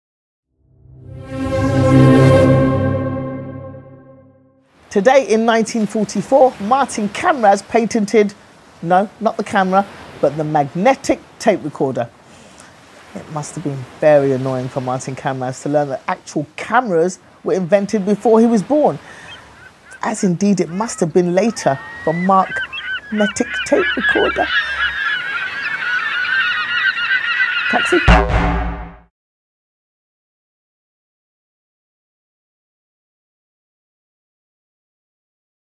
You what?!? Jun13: Gina Yashere
Gina's back with the origins of the world's most vital inventions. Today ...well guess what Martin Camras invented...

deliver a fact and a funny, in under a minute.